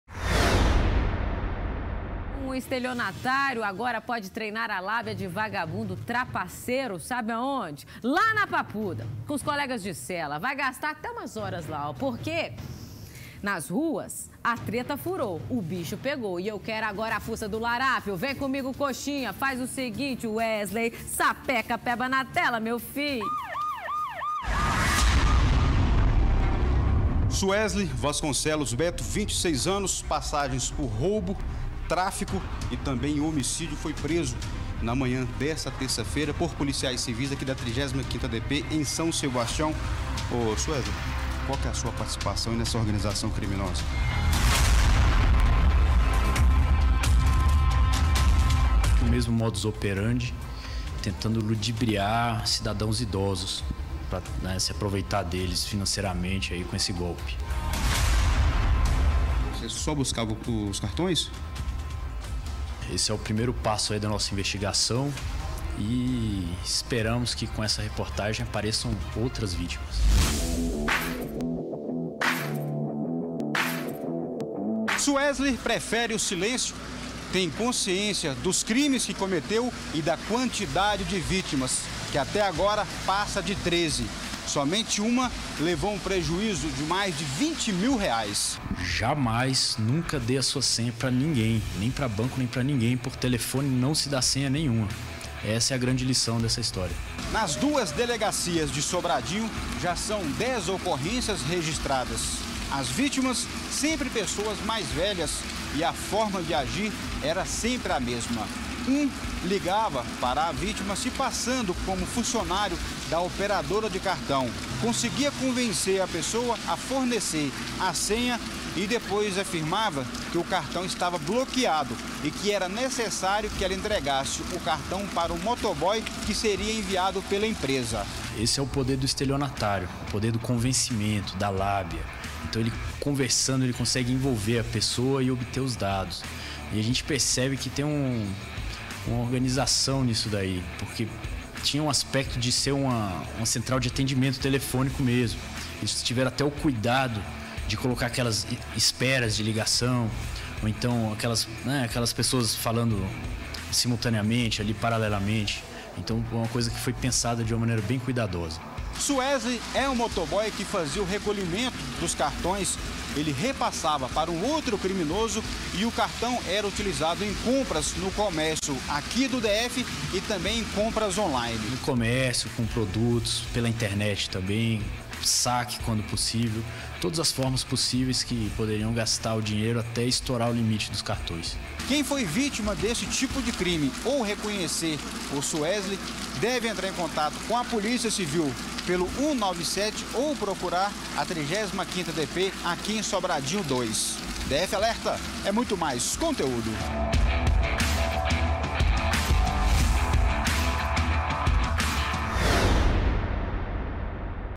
DF ALERTA - Cuidado! Golpista finge ser de operadora de cartão de crédito
27/03 - Golpista finge ser de operadora de cartão de crédito.